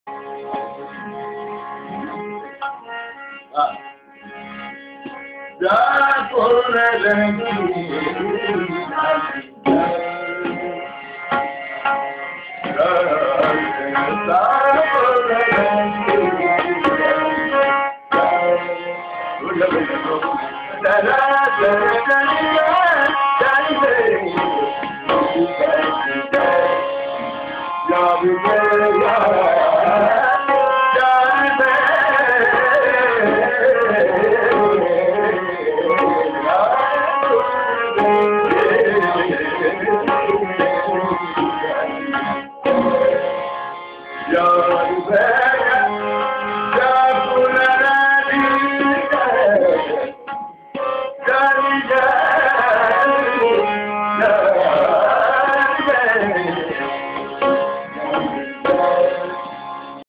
Pt. Mani Prasad - Raga Khamaj Tappa
Pt. Mani Prasad is a noted and well renowned exponent of the Kirana Gharana. He has invented many new ragas and composed bandishes for many existing ragas under the name of Dhyan Rang Piya.

In this recording, Panditji is singing a Tappa in Raga Khamaj.

He is accompanied by Ustad Fayyaz Khan on the tabla.